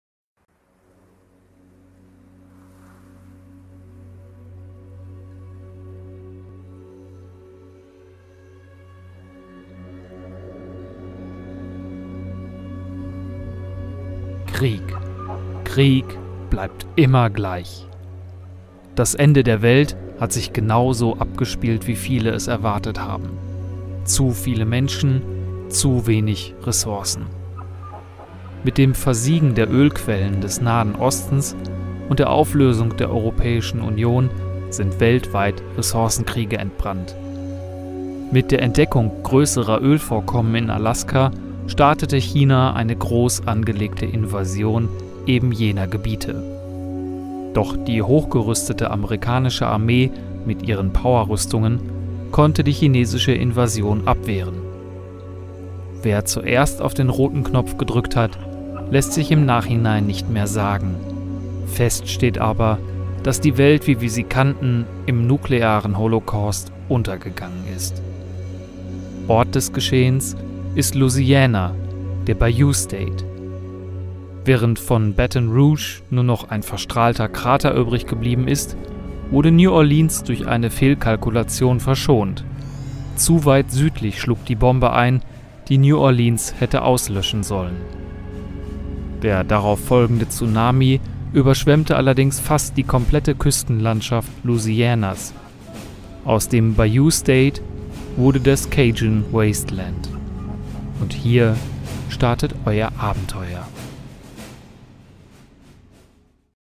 Fallout New Orleans - Intro für Pen & Paper RPG
Ein kleines Introvideo welches ich für die bald startende Fallout RPG Runde in und um New Orleans herum starten wird.